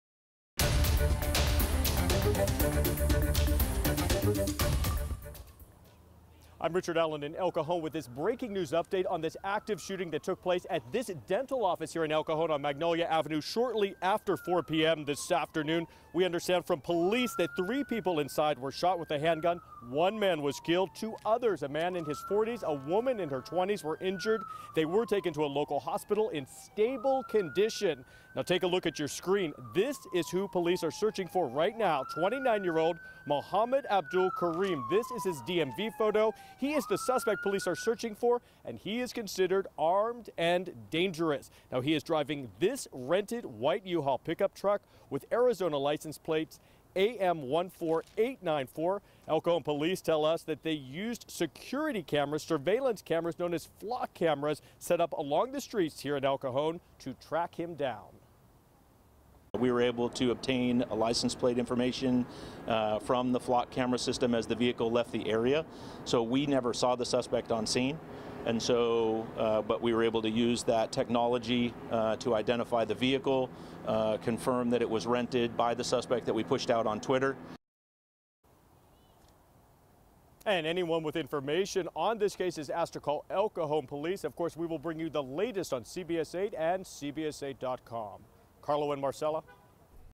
Suspect at large in deadly El Cajon shooting
One person is dead and two more were injured in an "active shooter event" in El Cajon Thursday, El Cajon police said. The two other victims were transported to the hospital.

Police responded to the shooting at a dental office on the 400 block of North Magnolia Avenue shortly after 4 p.m. Multiple police cars were on scene as officers searched the area.